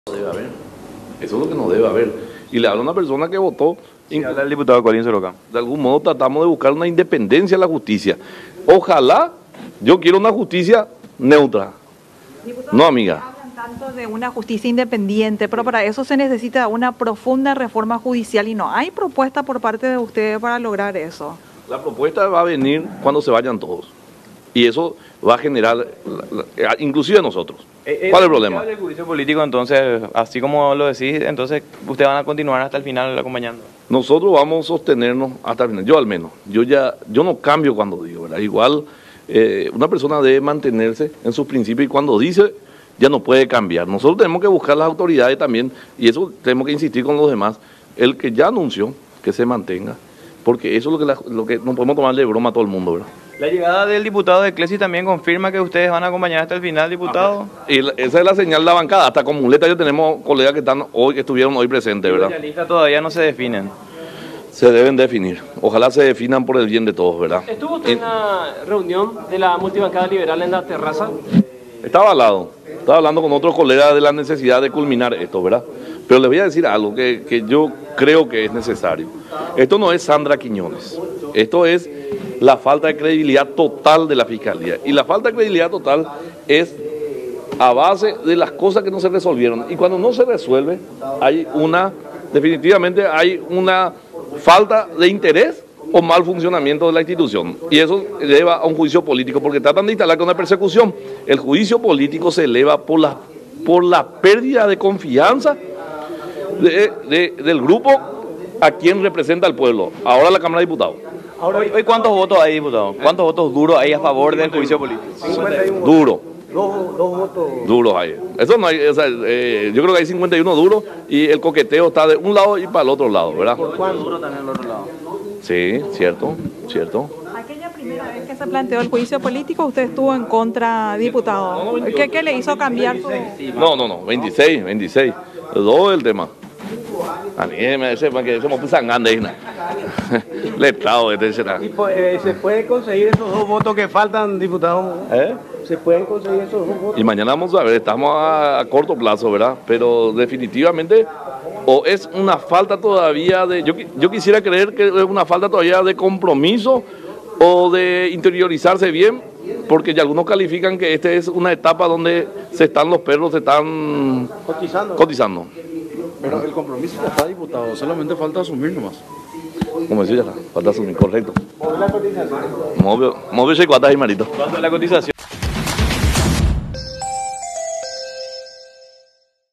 Siguió el debate por el juicio político
Nuevamente los diputados aprobaron un cuarto intermedio y el debate sobre el pedido de juicio político a la Fiscal General del Estado, Sandra Quiñónez, se retomará mañana a las 14 h.